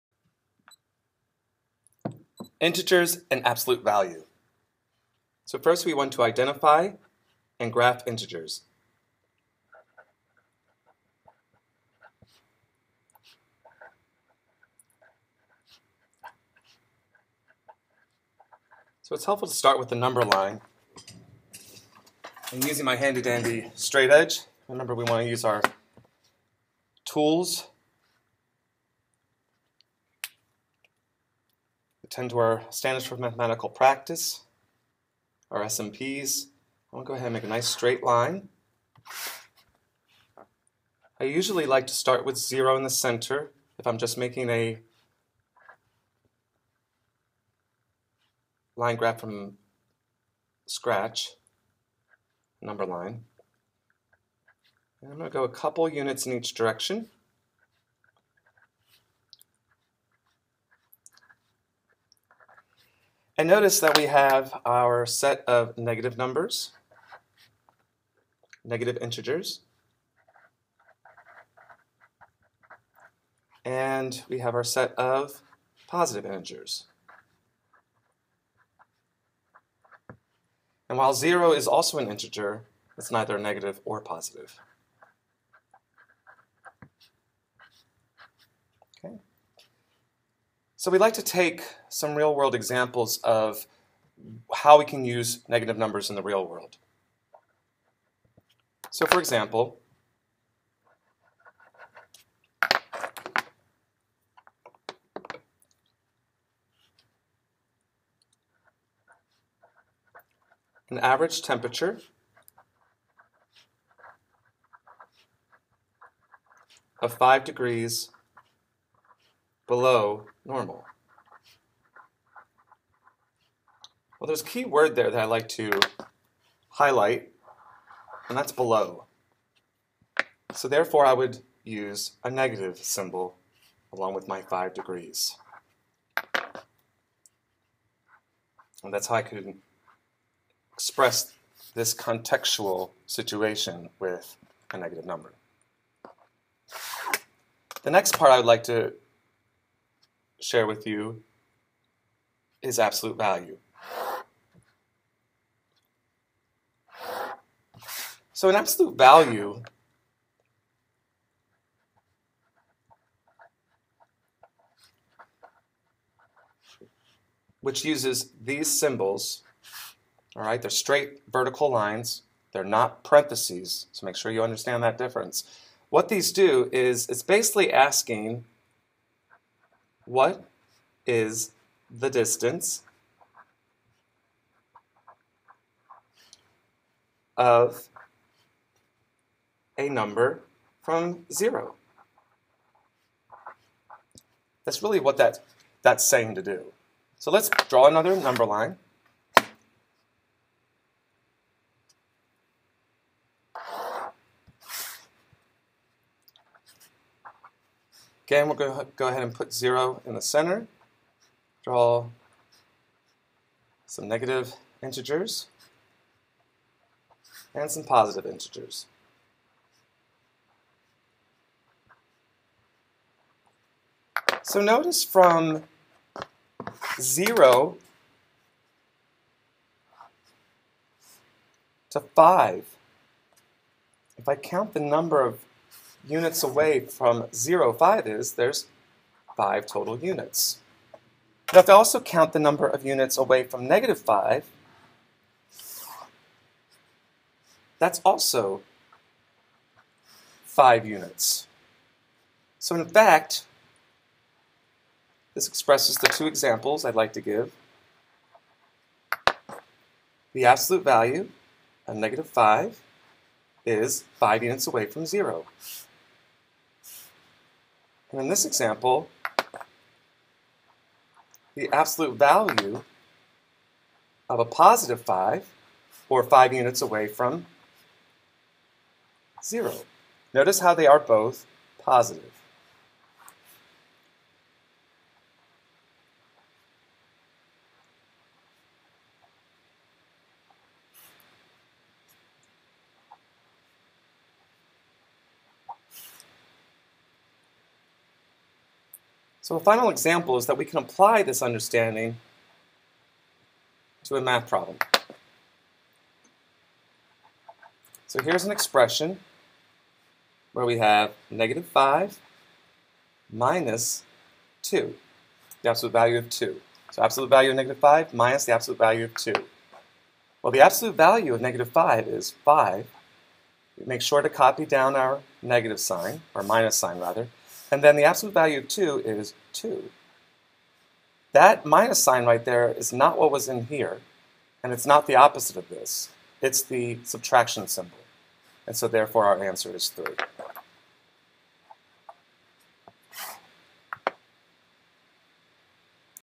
M7U1T1L1- Absolute Value & Integers
Prerequisite for 7.NS.3
How can absolute value be used to describe integers on the number line?